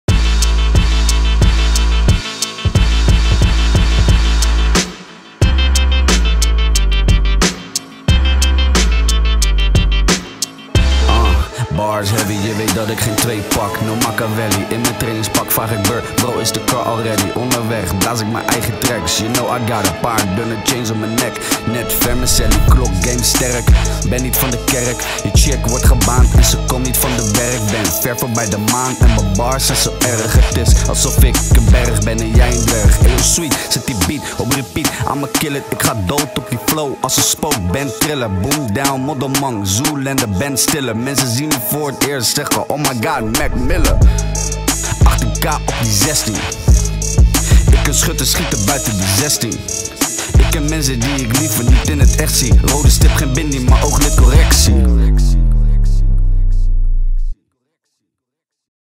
H3C HARD BARS 30: Boom Down
Een van de meest productieve Haagse MC's op dit moment...Boom Down. Straight Outta Skiffa brengt ie zijn bars langs om een sessie te knallen!

Wil jij ook een sessie doen? Of opnemen in de studio of je tracks laten mixen en/of masteren? Dat kan! of